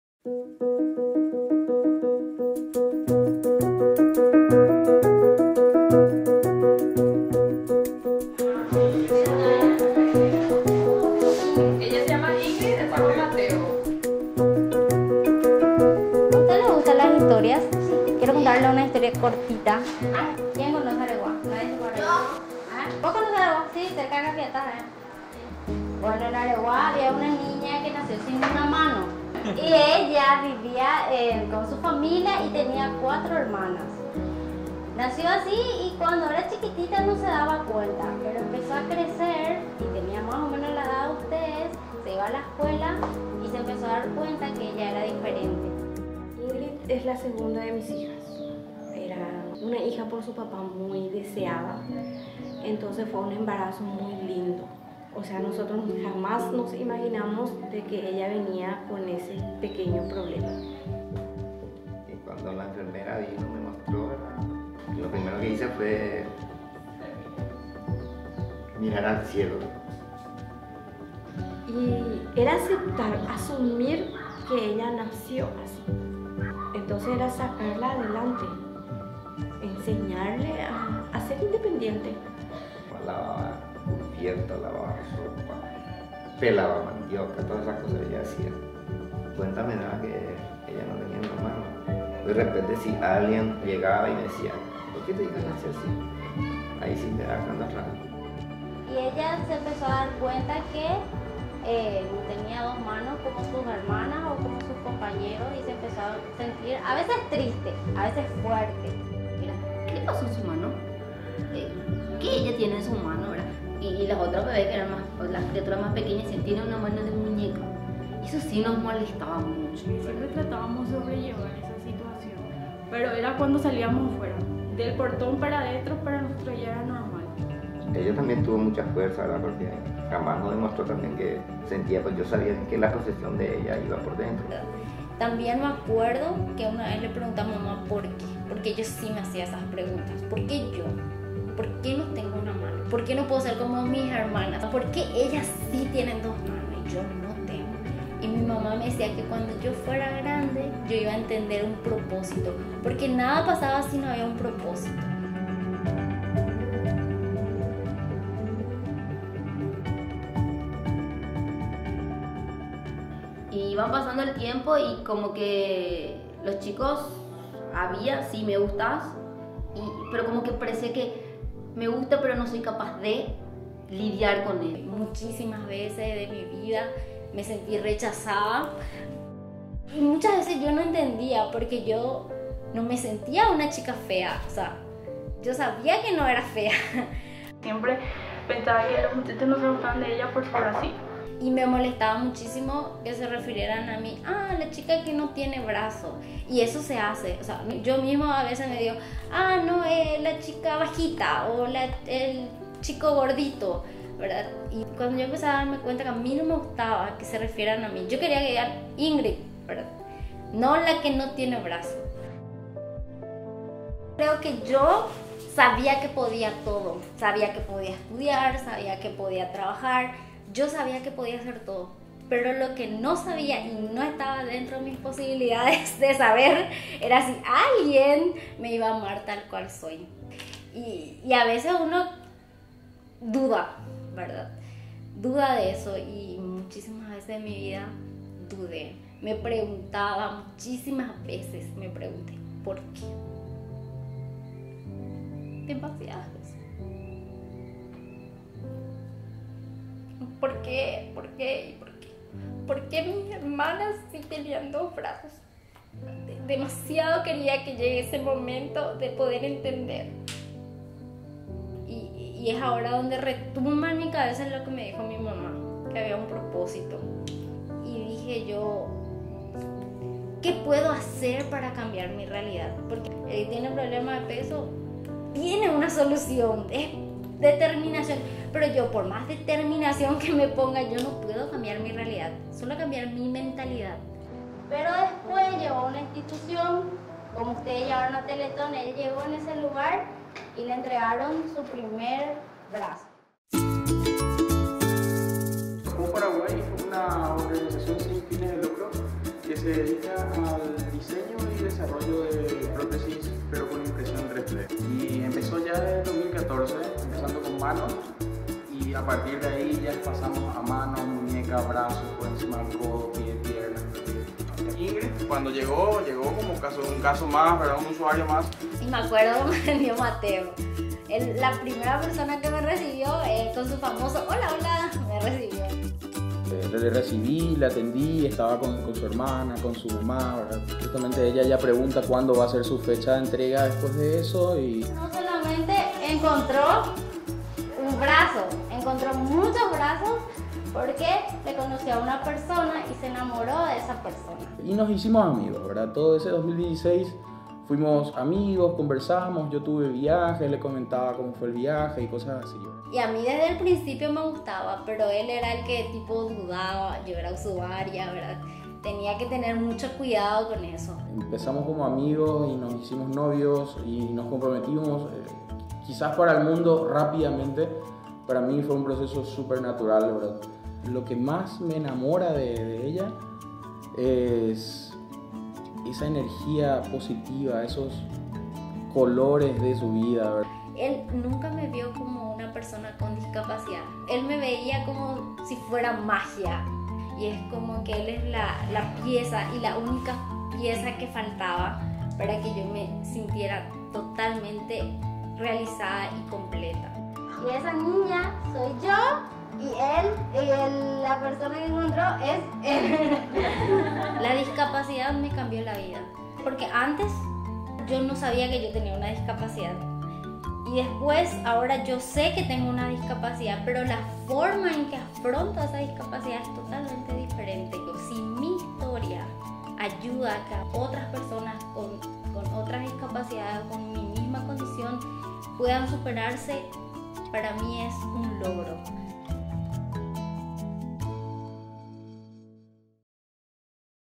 Ingrid Ortega - Historia de Vida 2018
“La discapacidad me cambió la vida, porque antes yo no sabía que tenía una discapacidad. Ahora yo sé que tengo una discapacidad, pero la forma en que afronto esa discapacidad es totalmente diferente.”.

Ingrid, es de Areguá y nos cuenta su historia. Creció con el amor de su familia y a lo largo de su vida fue lidiando con las barreras que se quería interponer en sus caminos.

¡Pero ella nunca se puso límites! Conocé su historia acá.